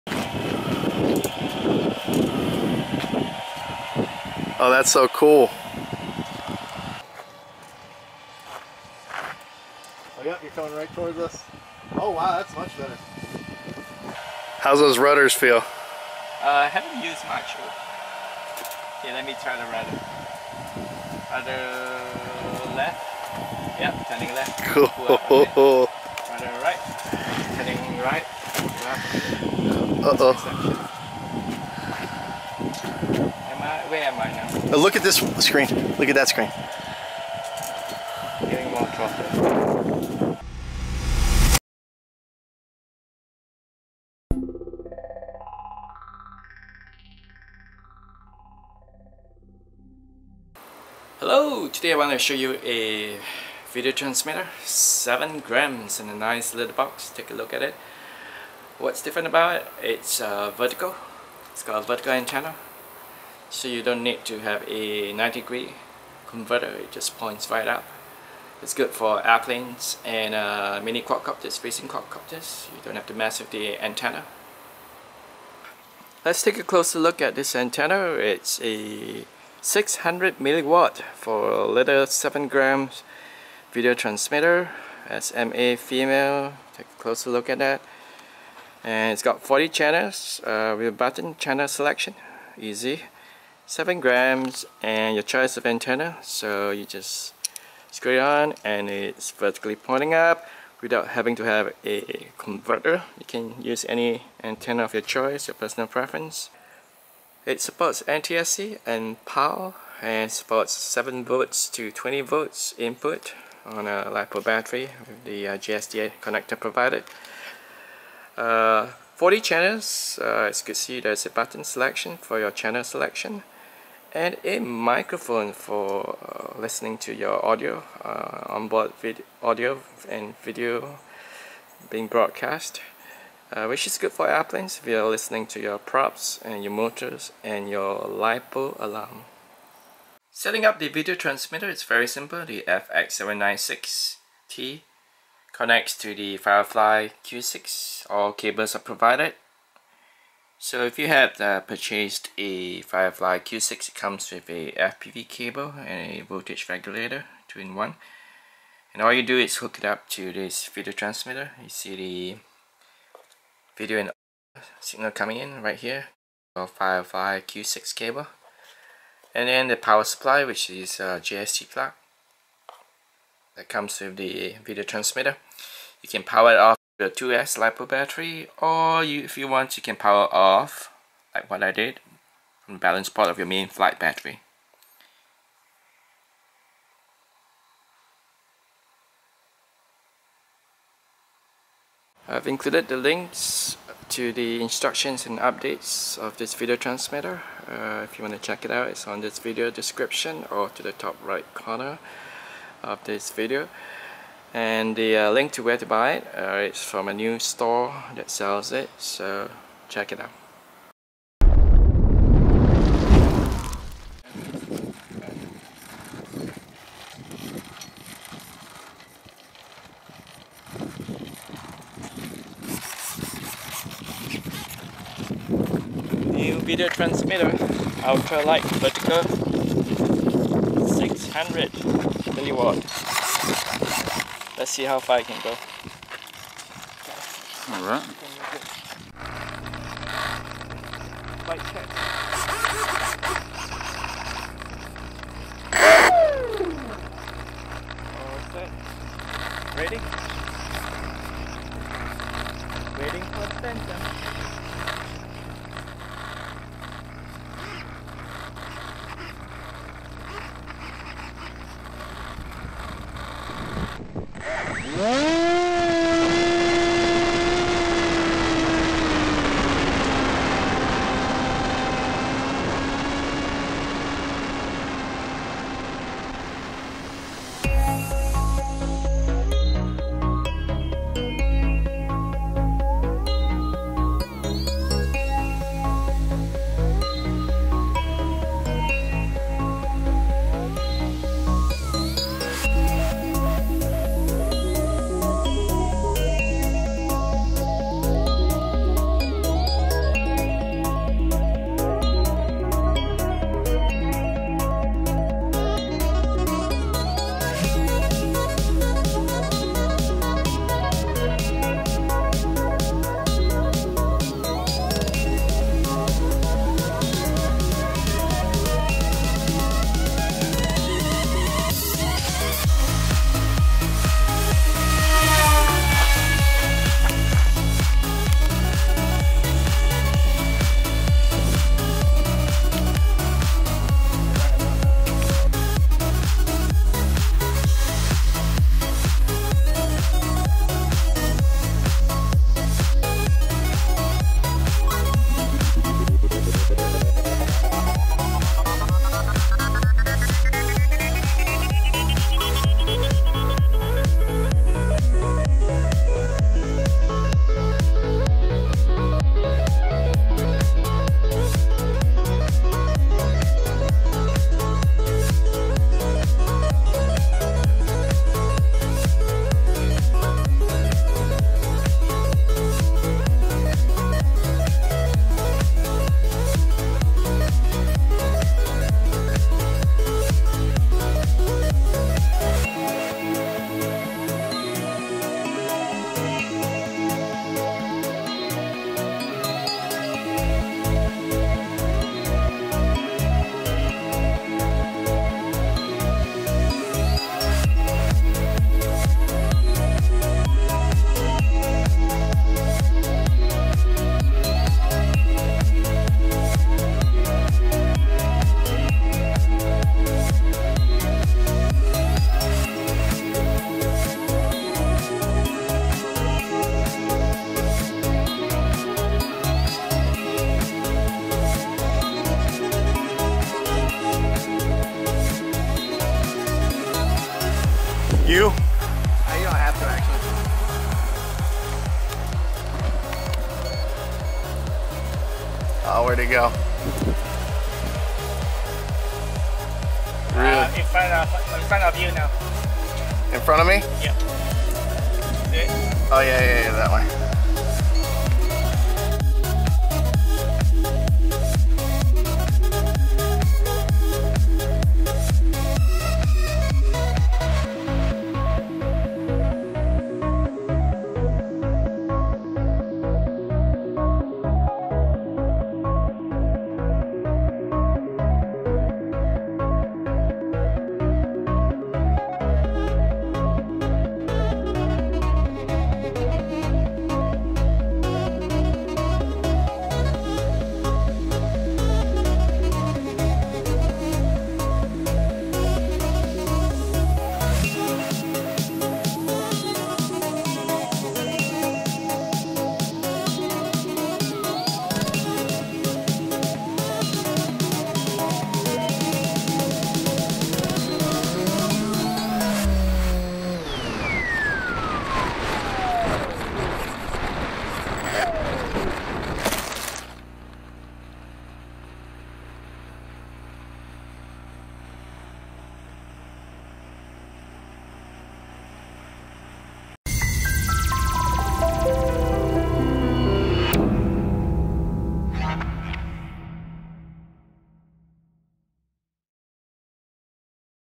Easy FPV setup Firefly Q6 FX796T
Setting FPV up with the Hawk-eye Firefly Q6 is easy. A fpv voltage regulator cable is included, for this example we use the FX796T 5.8G 600mW AV Transmitter. This 7 grams video transmitter features a vertical antenna with 40 channels and the operating voltage of 7-20 volts. Similar Mobius Mini, Runcam 2.

Starring: FX796T and Firefly Q6 v2.2
Producers: Leo, Lifish
Pilots: Leo, Chris, Ryan, Terry
Editorial: Kate,  Edy
Storyboard: Leo
Camera 1: Ryan

a contribution from the DroneMission.com Usability Test Team.  Share or Rave about it ...spread the word, welcome!